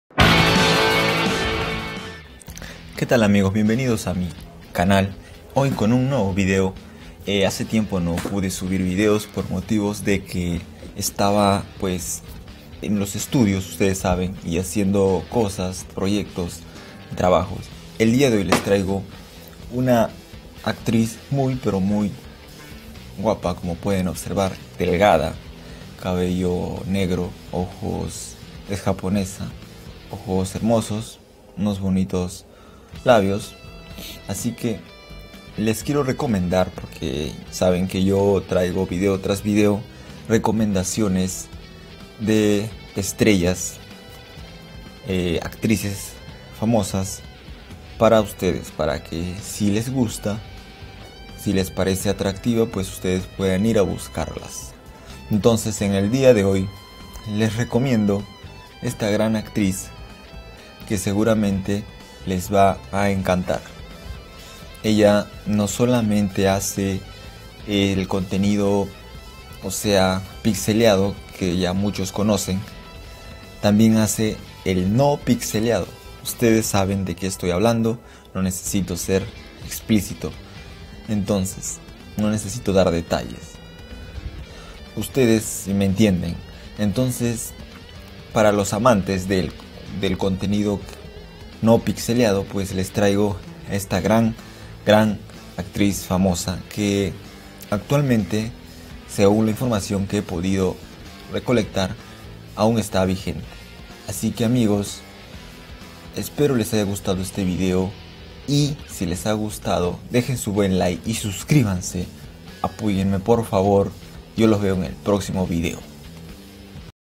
Satomi Suzuki
En este video hago una reseña de una actriz, será un explicación breve recomendandola para mis suscriptores que son seguidores de mi canal, espero les guste.